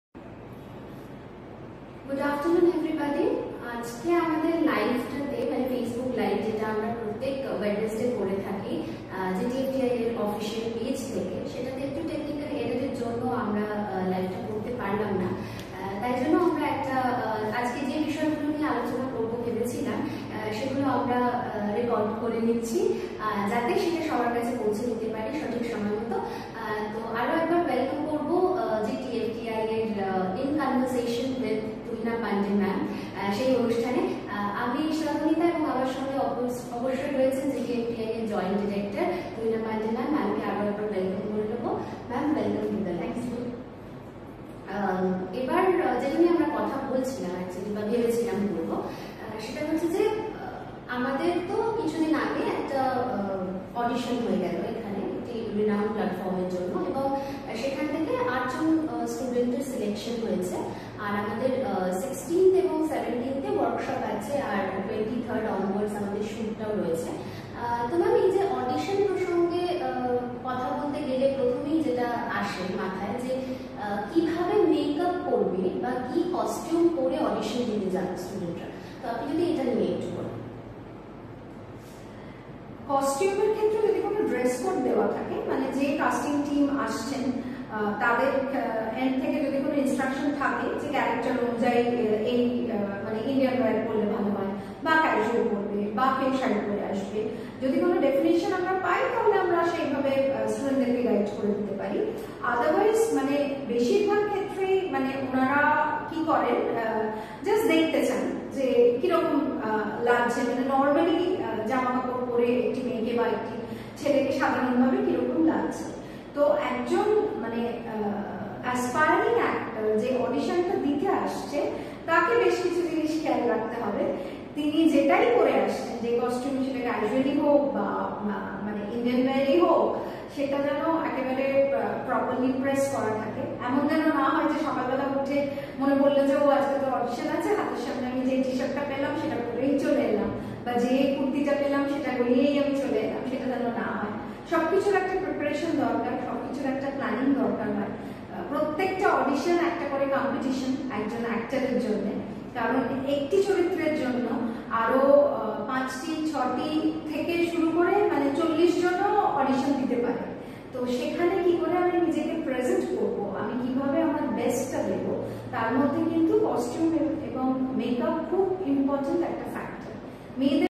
Discussion on auditions and importance of making a portfolio - "In Conversation with Tuhinaa Pandey"
"In Conversation with Tuhinaa Pandey"

Discussion on auditions and importance of making a portfolio.